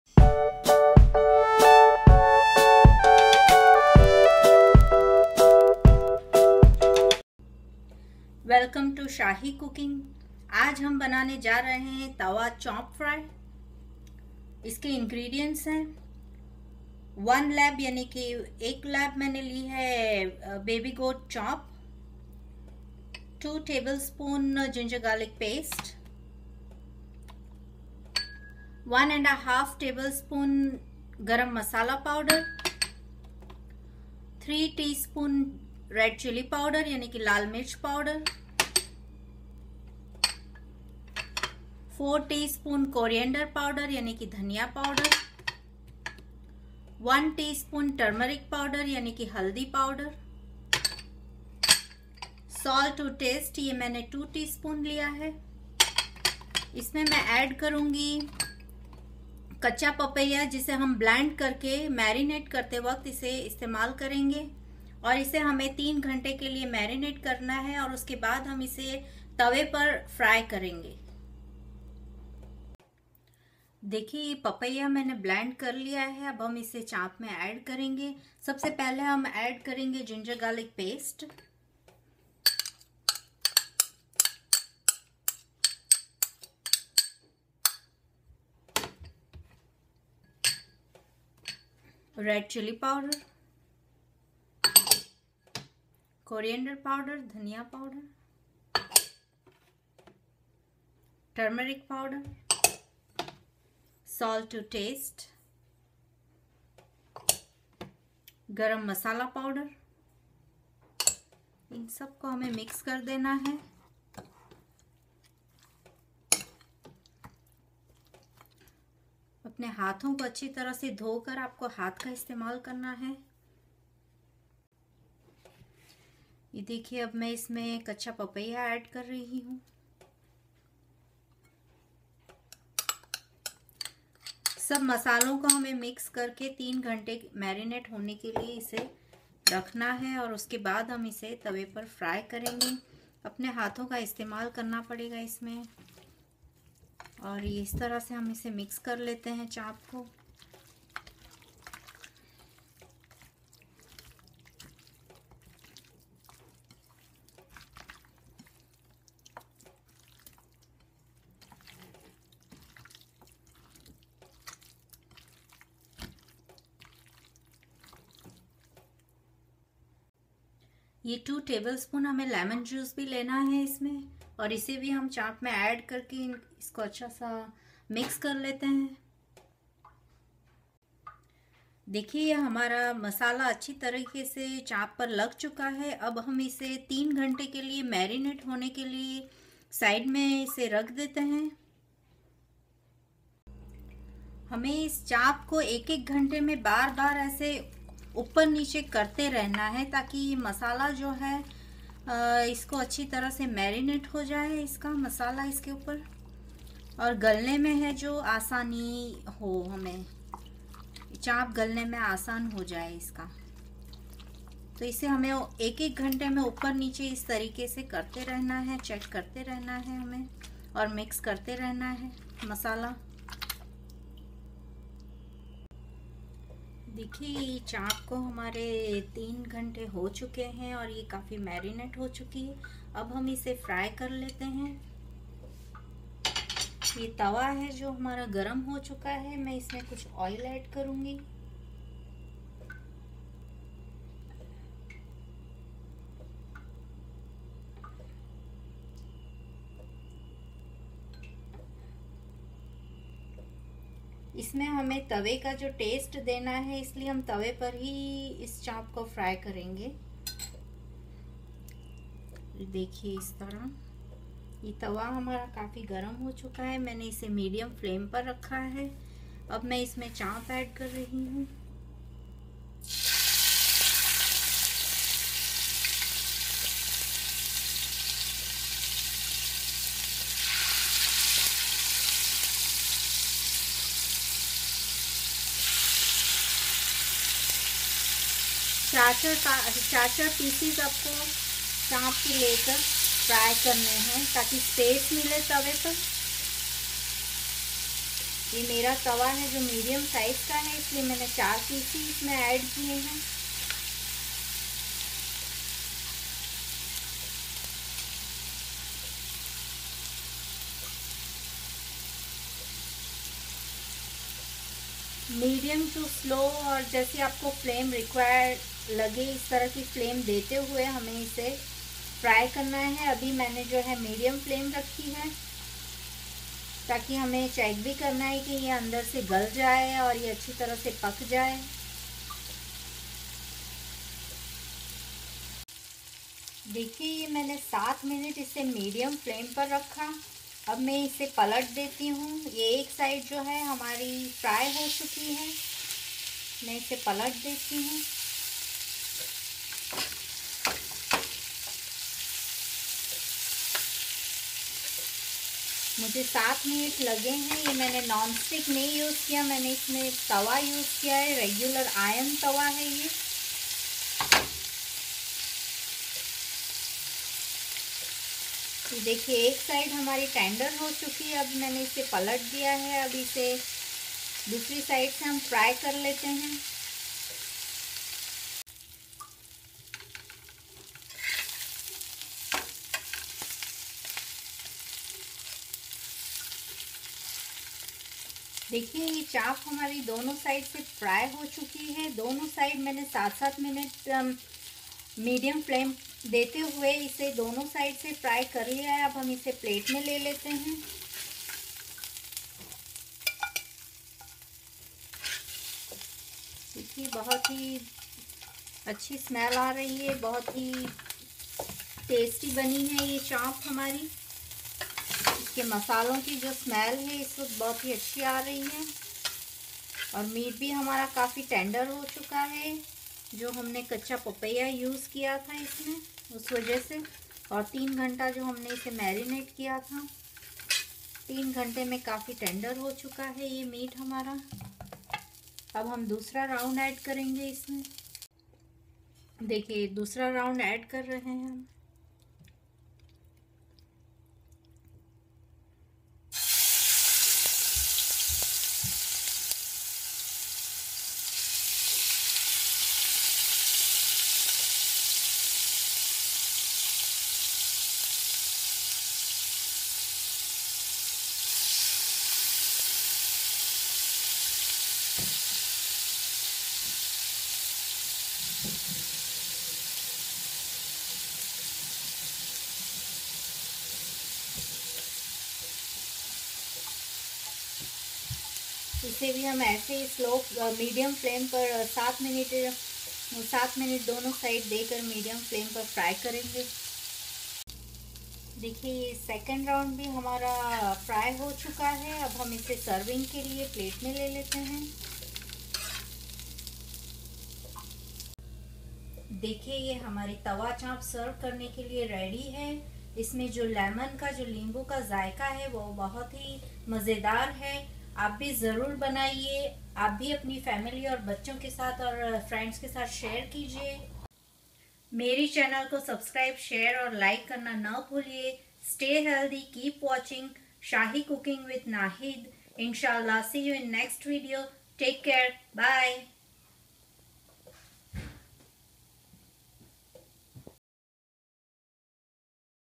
Street Food Tawa Chop Fry II How to Make Tawa Chops Fry ( Street Food Style)@shahicookingwithNahid
Popular Uploads/ Kabab/keema Kabob/chicken tikka/tikka Boti/Biryani/chicken Biryani/Tandoori/chicken Dana/Indian street food/Pakistani street food

Ingredients:
1LB. baby goat chop
3tsp. red chili powder
4tsp. coriander powder
1tsp. turmeric powder
salt to test
3tbs. ginger garlic paste
1.1/2 tbs. garam masala powder
small 1/4 raw papaya
2tbs. lemon juice

Preparation:
Blend raw papaya in a blender, and mix all above ingredients including papaya paste and marinate for about 3 or 4 hours, every 1-hour check chop and mix well by using hand. After 3 or 4 hours shallow fry on medium flame and serve with Naan, Chapati, etc. Enjoy with your family and friends. "Happy Cooking".